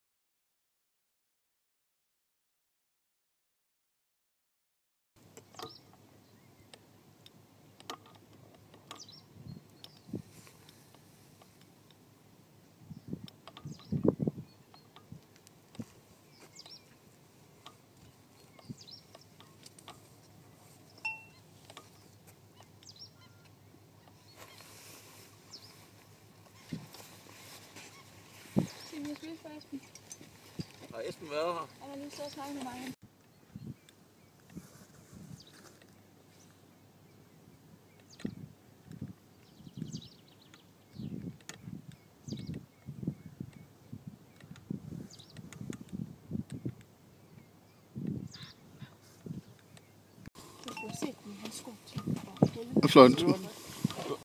Long-billed Dowitcher at Bøjden Nor
Long-billed Dowitcher, Limnodromus scolopaceus, at Bøjden Nor, 17 April 2013